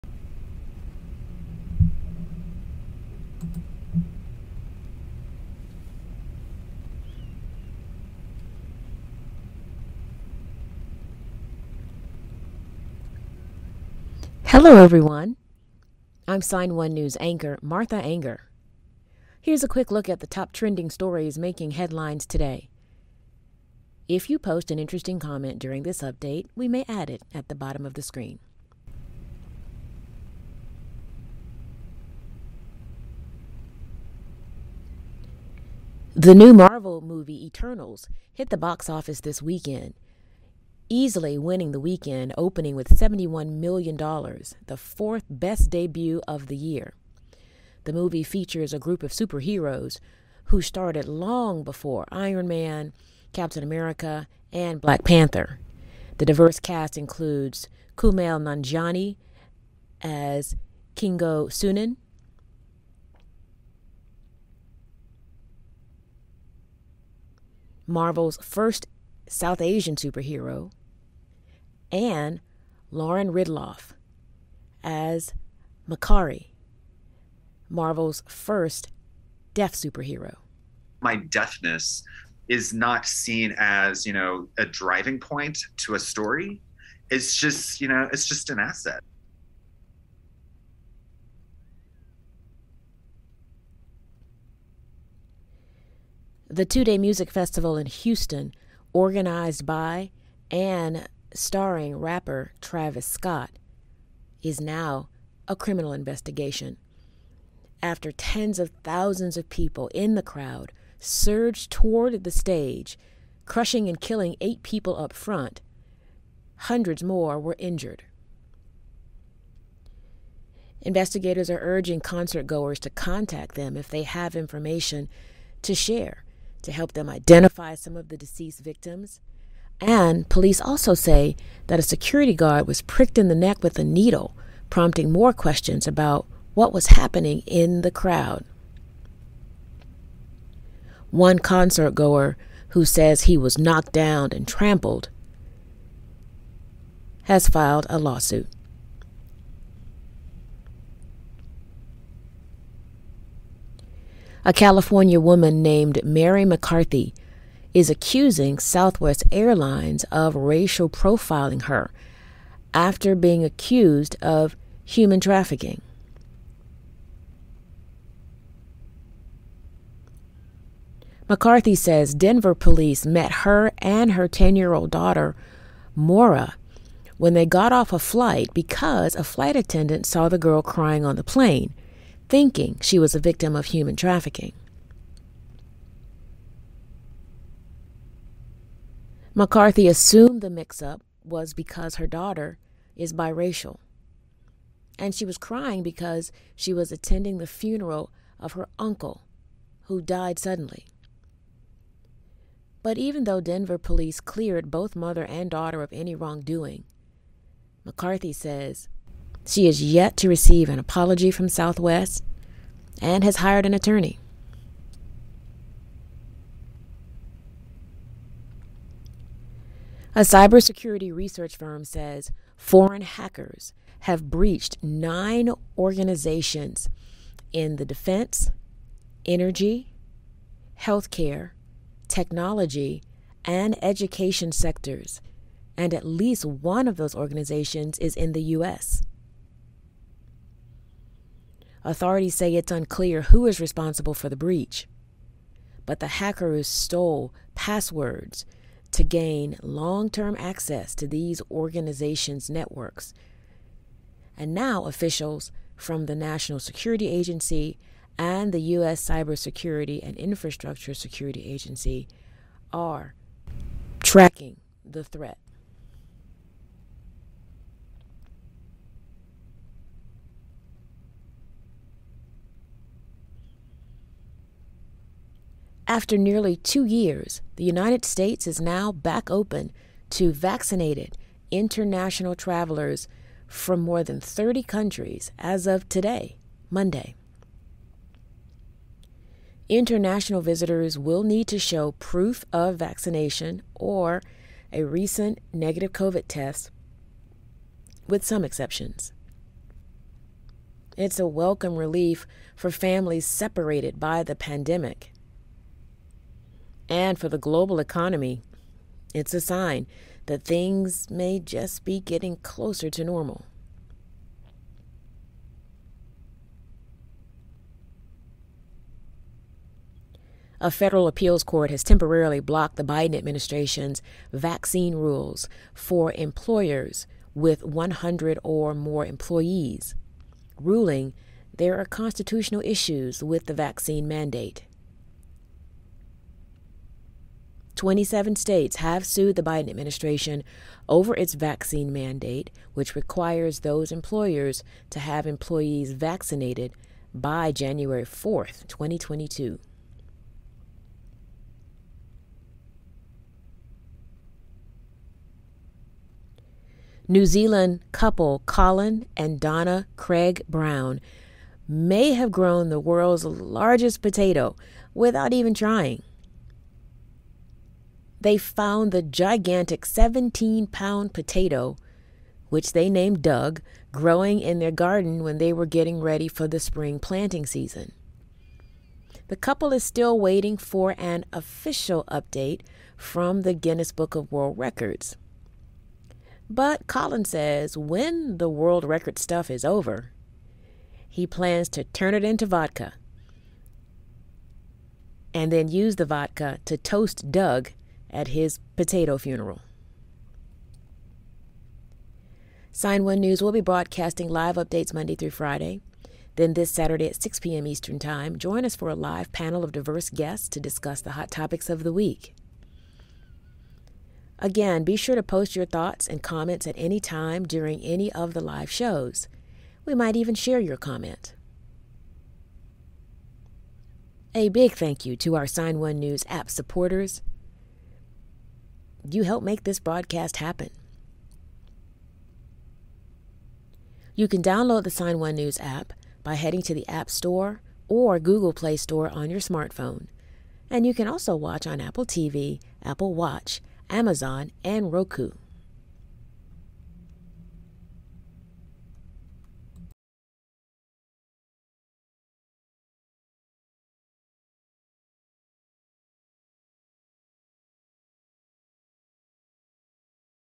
Sign1News Daily Update (11.8.21)
Today's Headlines

-Marvel's "Eternals" debuts first deaf superhero
-Latest information on deadly crowd surge at Astroworld
-Woman accused of human trafficking bi-racial daughter
-Hackers breach multiple US defense contractors
-Vaccinated travelers allowed to travel to the US
-Federal court blocks vaccine mandate for private companies
-Couple grows what may be world's largest potato